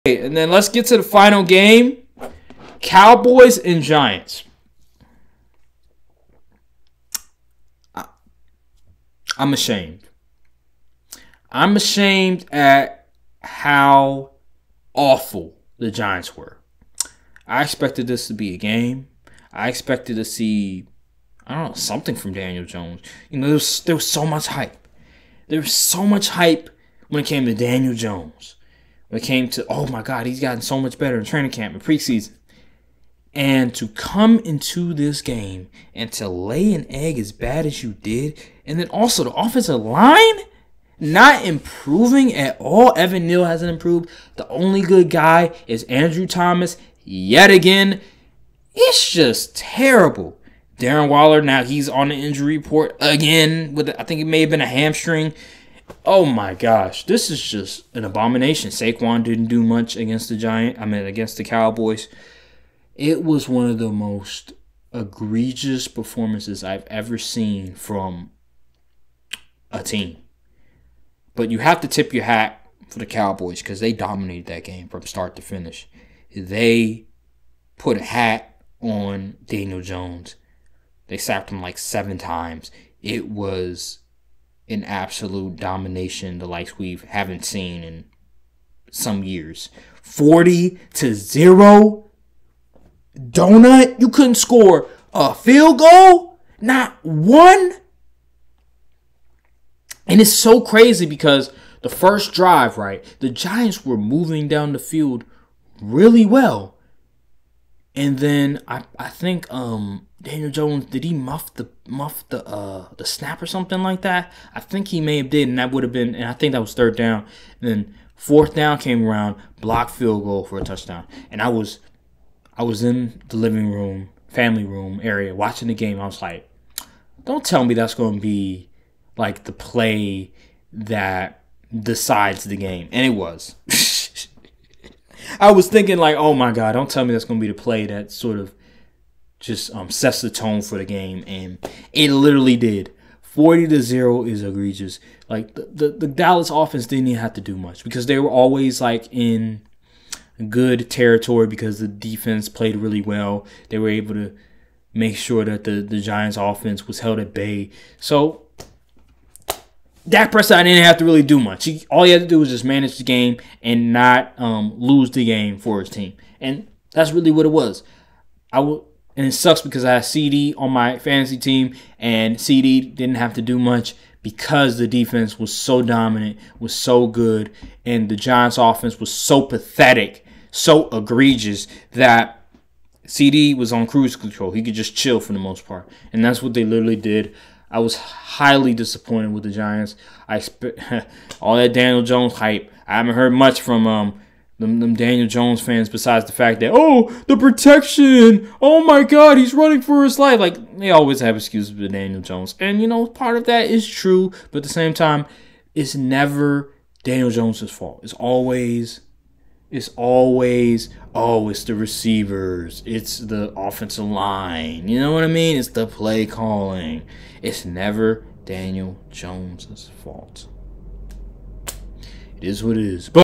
THE GIANTS TRICKED US! | Dallas Cowboys vs New York Giants Reaction | Courtside View Podcast
Aaron gives his thoughts on the Cowboys shut out victory vs the Giants. Are the Cowboys that good or the Giants overrated? Who's to blame for the Giants performance? Are the Cowboys the team to beat in the NFL? Find out in today's video.